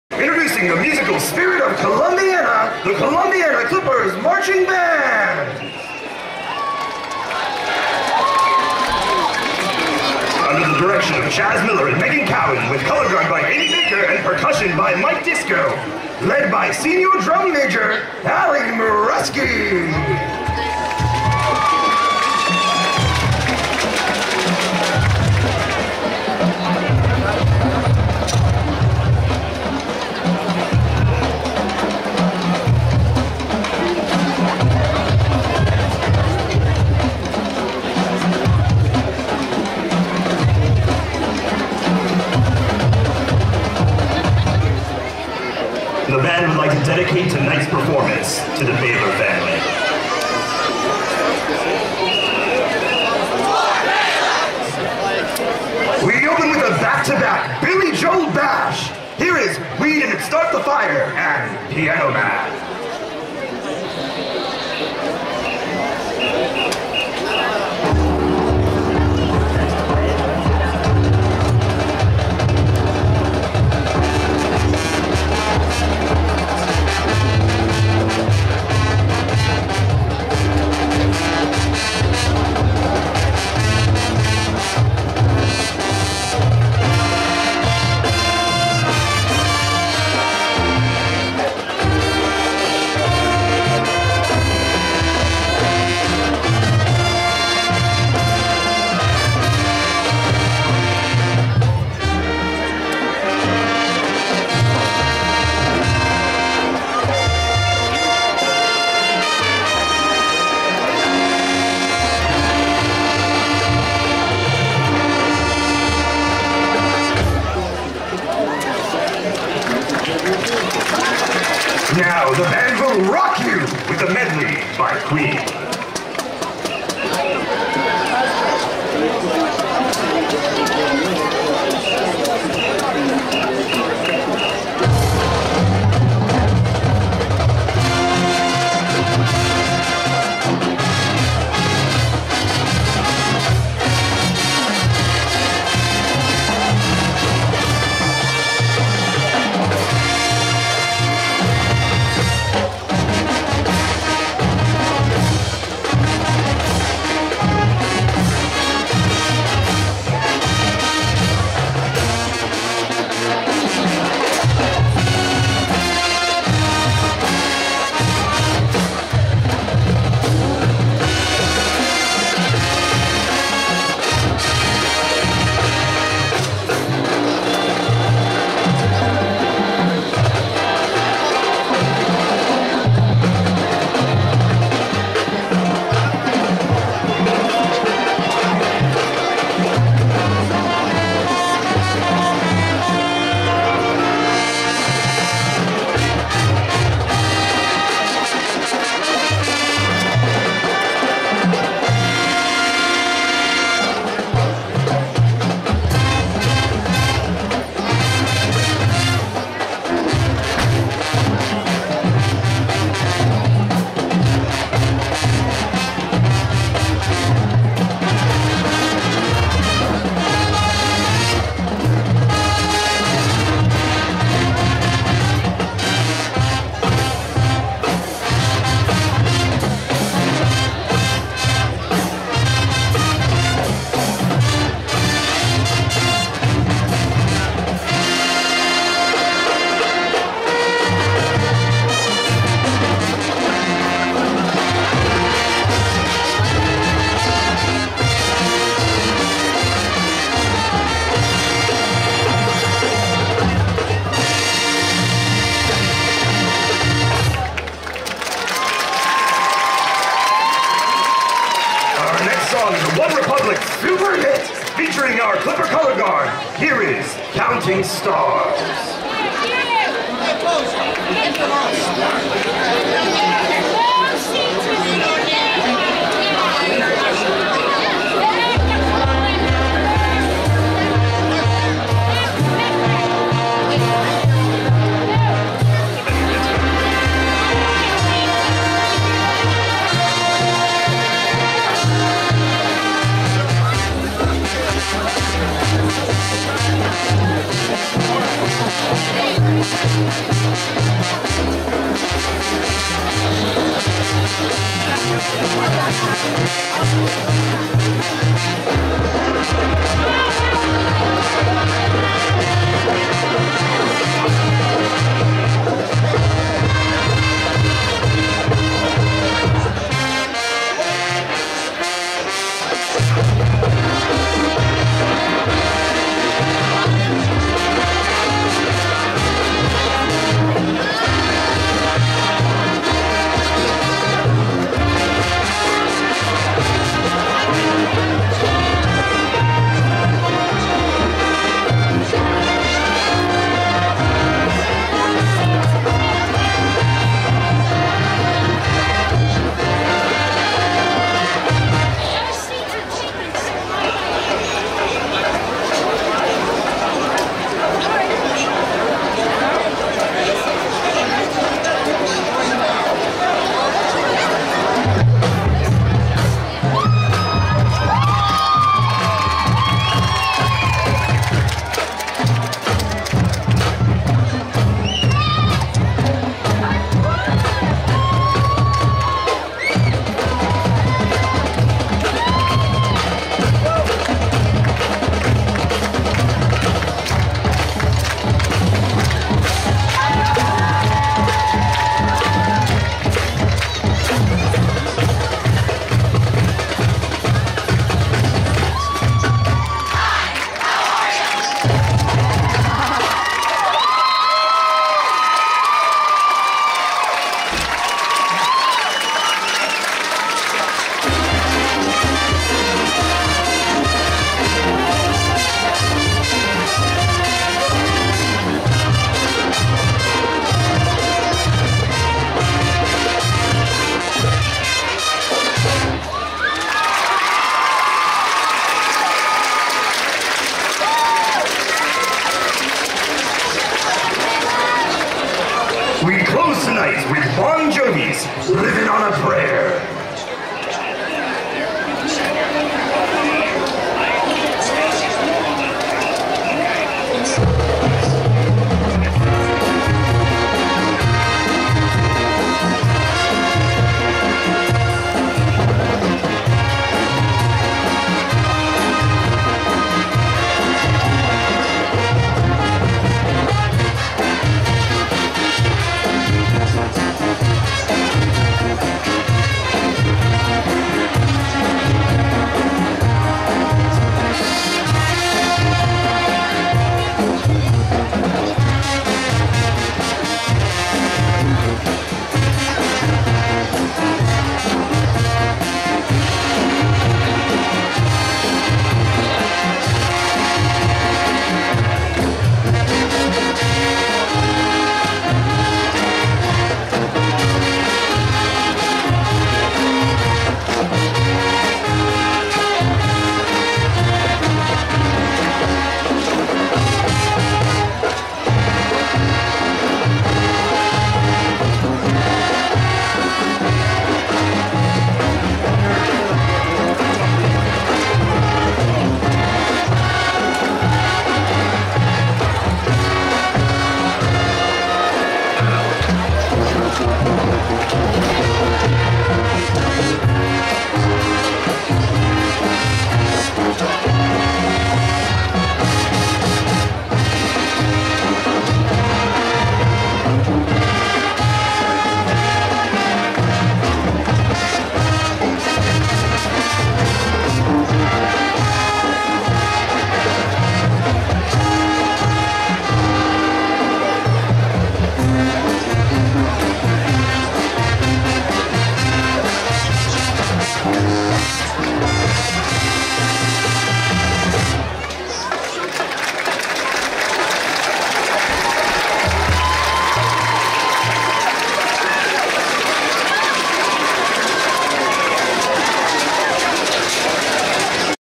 Columbiana High School 2015 marching band Sept. 4th halftime show
2015 Columbiana High School marching band - Band Dance